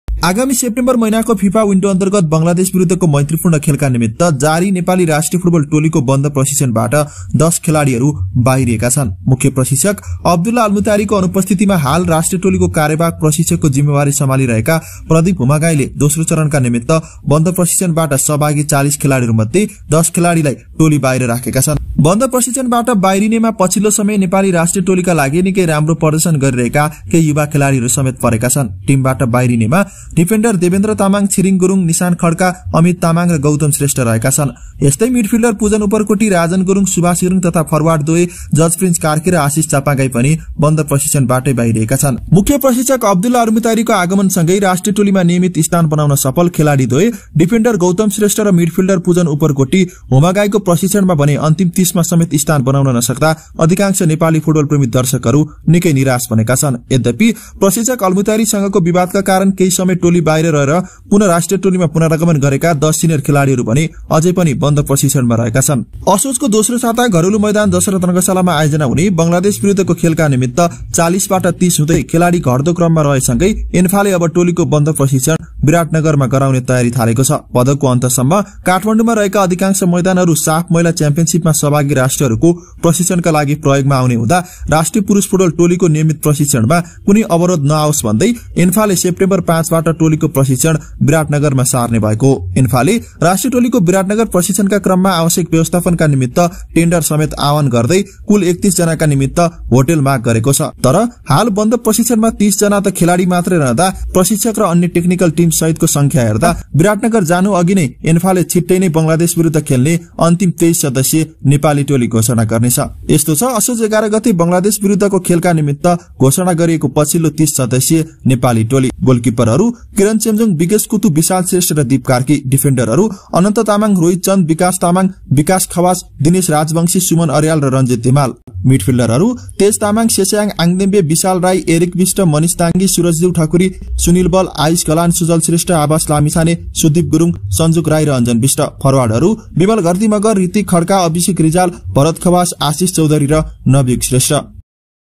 Pujan र Gautam किन परेनन् Humagainको अन्तिम ३०मा ? Closed Camp अब Biratnagarमा |Nepal vs Bangladesh
Nepal has reduced their players numbers to 30 from 40 for the upcoming friendlies against Bangladesh. Some of the young players who have been performing very well for the national team in the recent past have also dropped out of closed training.

The defenders Debendra Tamang, Chiring Gurung, Nishan Khadka, Amit Tamang and Gautam Shrestha have left the team. Similarly, midfielders Pujan Uparkoti, Rajan Gurung, Subas Gurung and forward George Prince Karki and Ashish Chapagai have also left the closed training.

Photos & video credit in this video:ANFA, Iran FA,Gautam Shrestha FB,Pujan Uparkoti FB